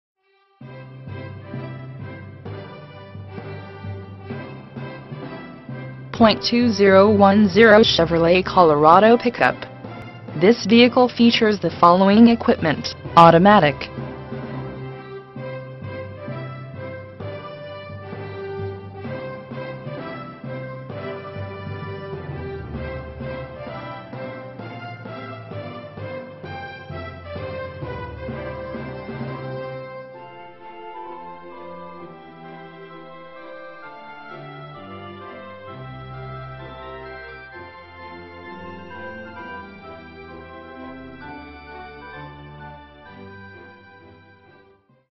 2010 Chevrolet COLORADO - Cals Park -n- Sell - Anchorage, AK 99518
900 E. Dowling Rd

For Sale: 2010 Chevrolet COLORADO
VIN: 1GCJTCDE4A8139182
Engine:
Drivetrain:
Transmission: automatic
Mileage: 24,318
Color: RED (exterior)  (interior)

Features:
2010 Chevrolet COLORADO Pickup. This vehicle features the following equipment: automatic, ,